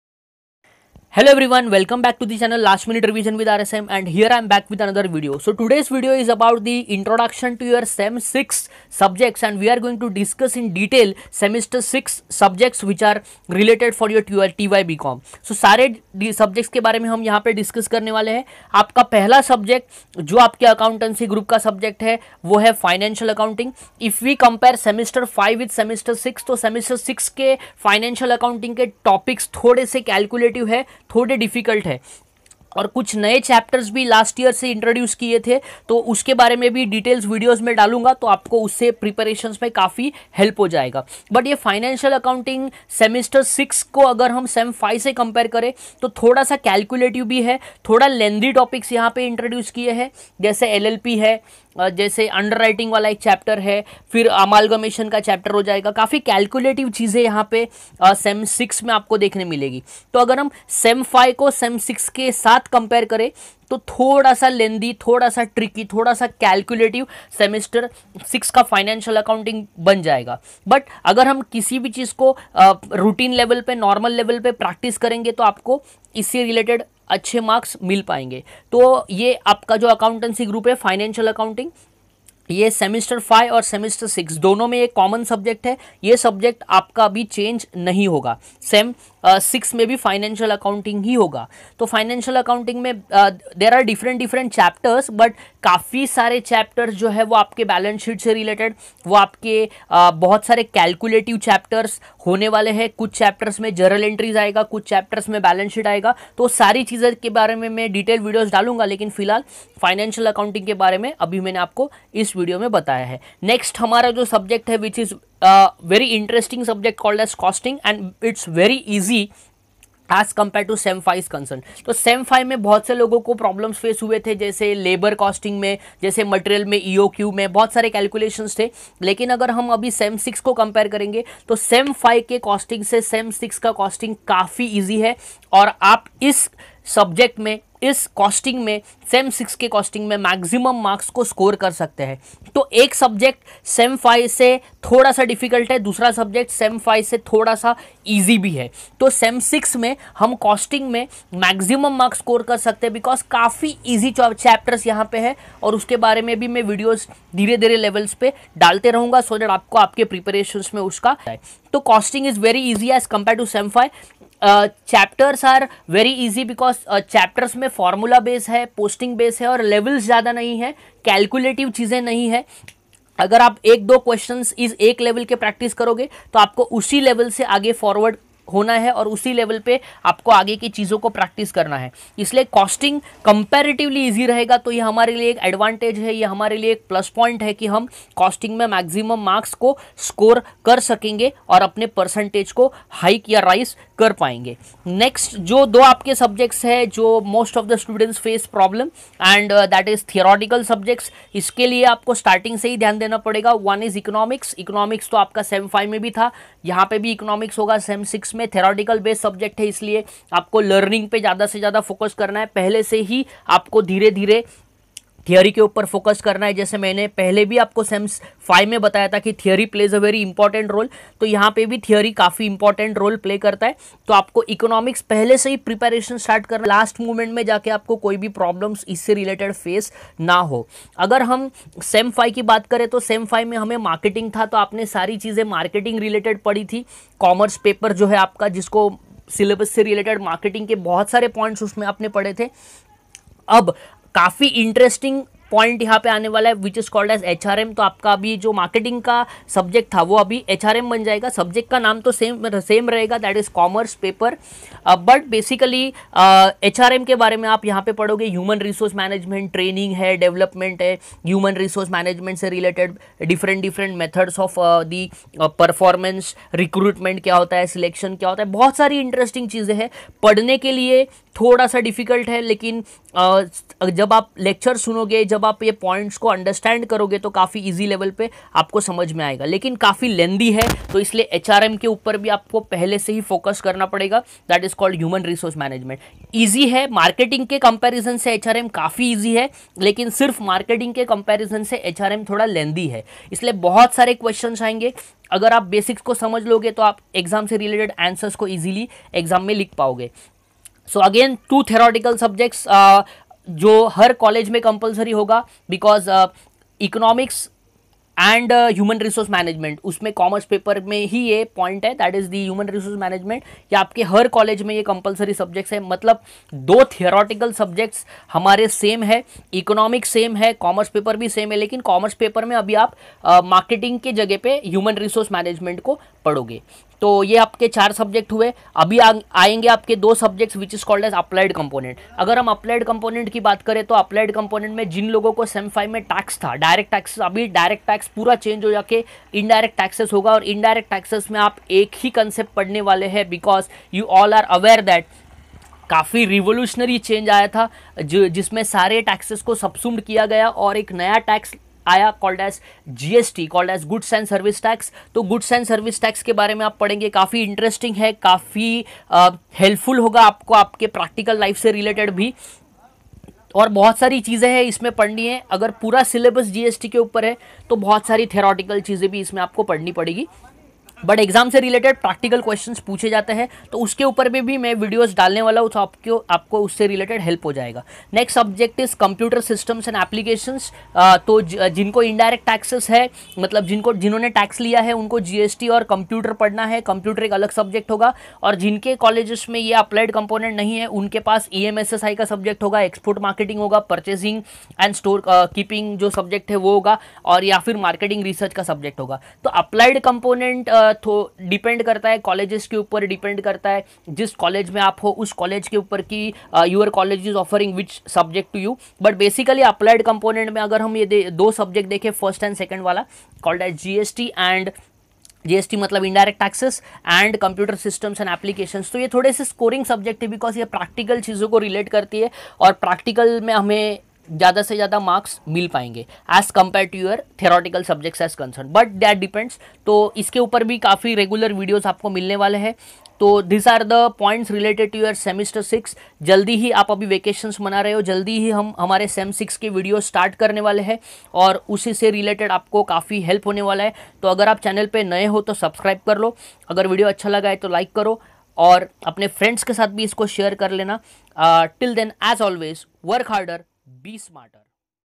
TYBCOM SEM 6 INTRODUCTION SEM 6 ALL SUBJECTS
Hello friends in today's video I will give you introduction to subjects of SEM 6